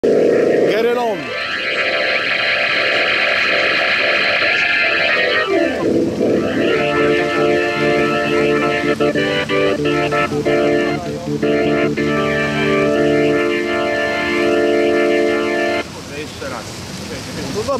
Unknown musician performing on hammond organ xk-1
Wroclaw, Poland - May, 2015 Same artist in awesome performance You will not regret it!!!